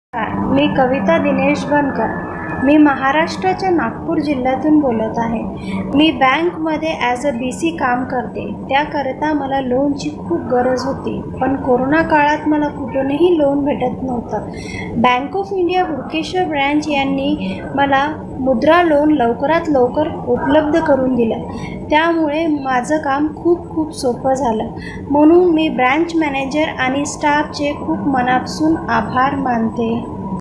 Mudra Loan Approved by Bank of India during Corona Covid 19 Period - Customer Testimonials #250
Ms. Kavita Dinesh banker from Nagpur, Maharashtra.
She got a loan from Bank of India during corona period.
She is grateful to entire banker staff.